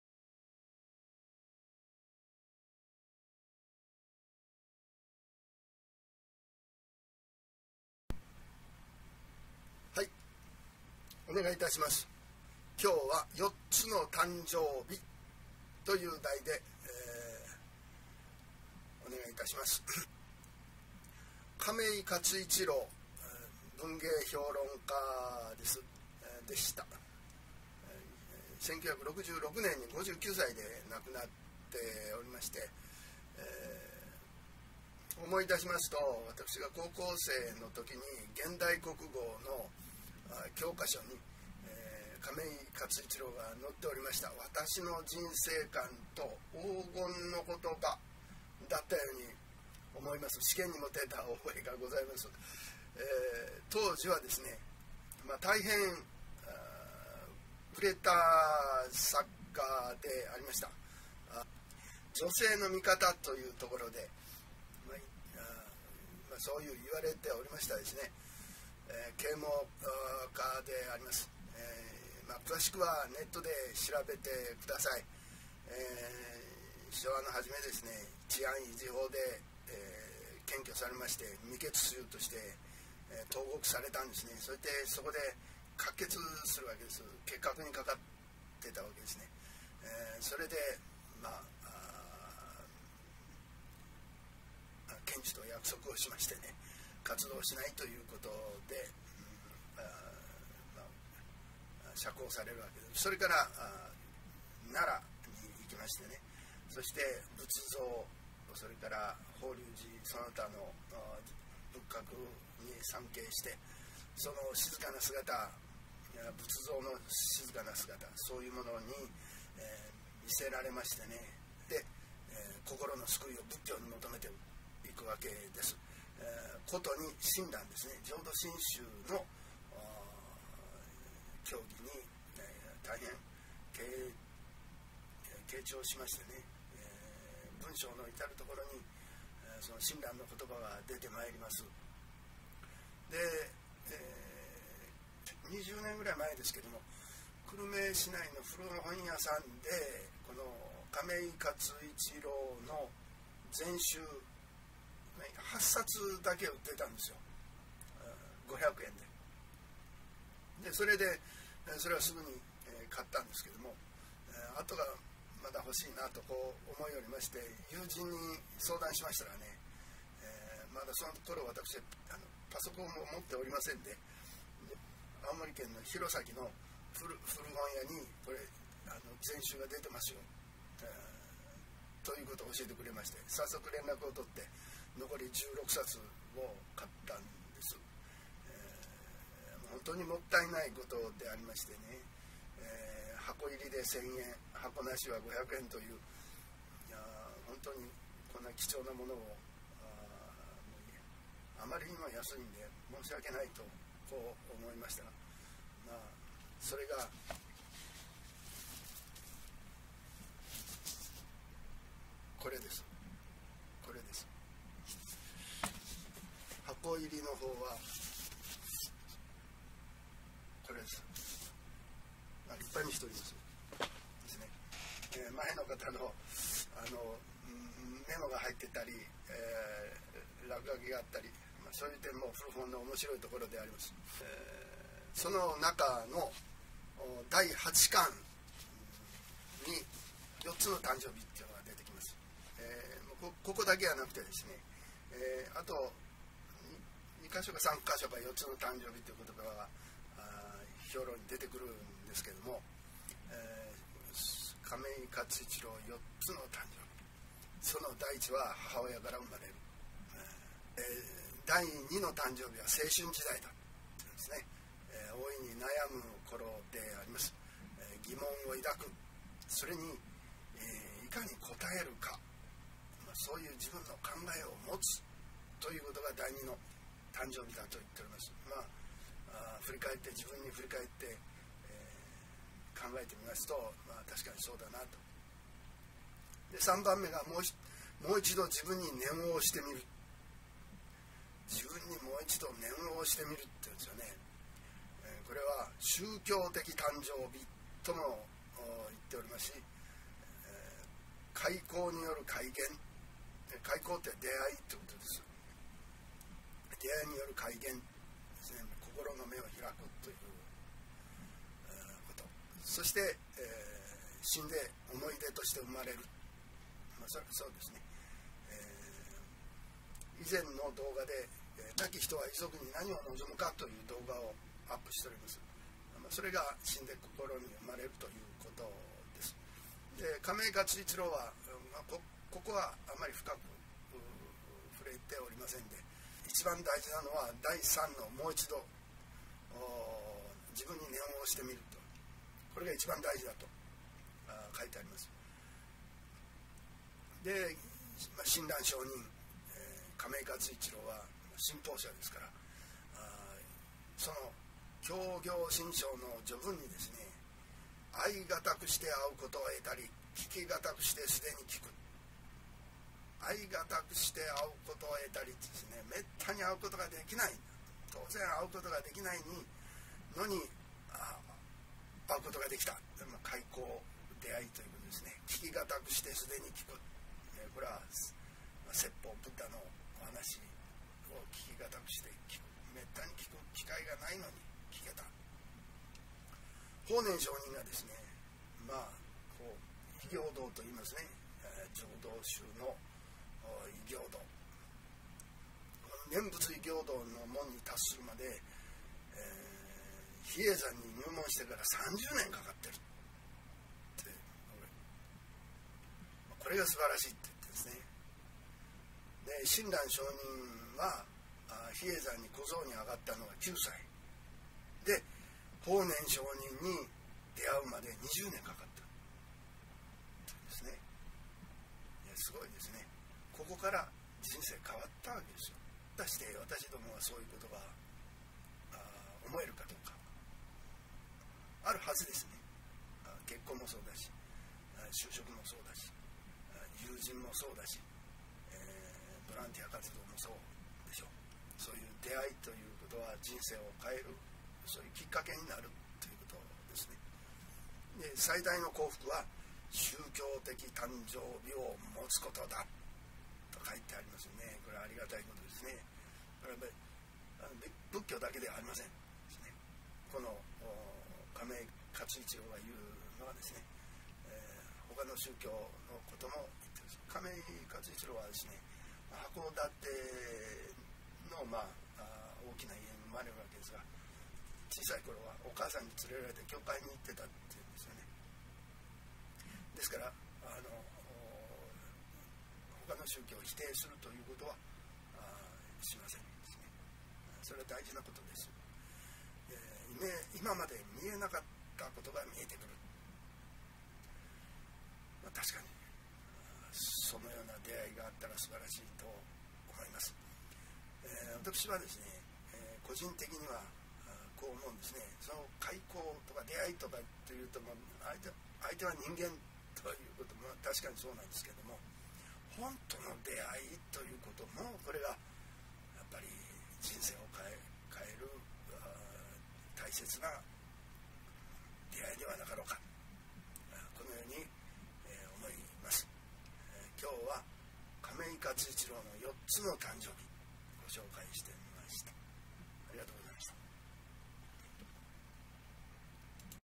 四つの誕生日
〇亀井勝一郎（1907～1966）全集（講談社1972）との出会い
・ 高校生のとき現代国語の教科書に「黄金の言葉」がのっていた。
・ 久留米市内の古本屋で8冊購入。友人に頼んで弘前の古本屋を紹介して
　　もらい残り１6冊を購入。
・ 女性の味方、社会啓蒙家、執筆のバックグラウンドは仏教、親鸞。
・ こころの救いを仏教にもとめた。

〇四つの誕生日全集第８巻123～124ｐ.
・ 母親から生まれる
・ 青春時代。疑問をいだく。如何にこたえるか。
・ もう一度自分に念をおしてみる。宗教的誕生日。邂逅による開眼
・ 死んで思いでとして生まれる

〇最も重要なのは第三の誕生日
・ 親鸞聖人『教行信証』序に「遇い難くして遇うことを得たり、
　　聞き難くして已に聞く」
・ 法然上人が念仏易行の門に達するまでに、比叡山に入ってから３０年。
　　親鸞が法然上人に出会うまで２０年

〇最大の幸福は宗教的誕生日をもつことだ。
　今まで見えなかったことが見えてくる。
本との出会いの影響は大きい。邂逅は、本との出会いもある。